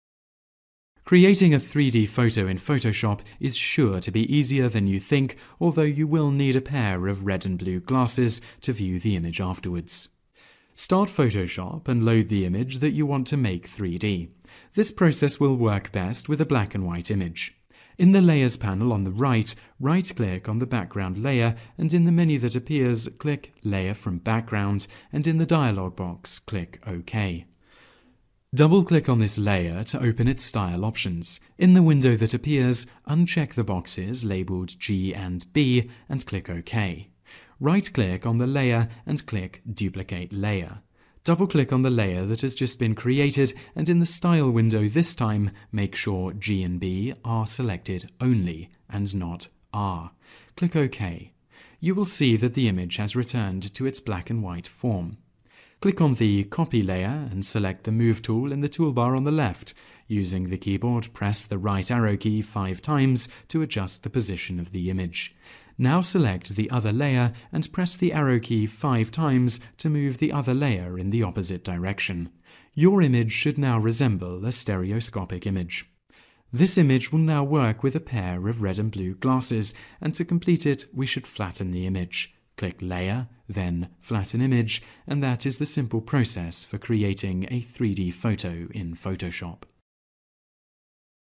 How to Make 3D Photo in Photoshop CS5
This tutorial will show you how to create a 3D photograph in Photoshop.

Creating a 3D photo in photoshop is sure to be easier than you think although you will need a pair of 'red and blue' glasses to view the image afterwards.

Step # 1 -- Creating an Adjustment Layer

Start Photoshop and load the image that you want to make 3D. This process will work best with a black and white image. In the layers panel on the right, right click on the background layer and in the menu that appears click "Layer from Background" and in the dialog box click "OK".

Step # 2 -- Creating the Colored Layers

Double click on this layer to open its style options. In the window that appears uncheck the boxes labeled "G" and "B" and click "OK". Right click on the layer and click "Duplicate Layer", double click on this layer that has just been created and in the style window this time make sure "G" and "B" are selected only, and not "R". Click "OK". You will see that the image has returned to its black and white form.

Step # 3 -- Splitting the Layers

Click on the "copy" layer and select the move tool in the toolbar on the left. Using the keyboard press the right arrow key five times to adjust the position of the image. Now, select the other layer and press the left arrow key five times to move the other layer in the opposite direction. Your image should now resemble a stereoscopic image.

Step # 4 -- Completing the Process

This image will now work with a pair of 'red and blue' glasses and to complete it we should flatten the image. Click "Layer" then "Flatten Image" and that is the simple process for creating a 3D photo in photoshop.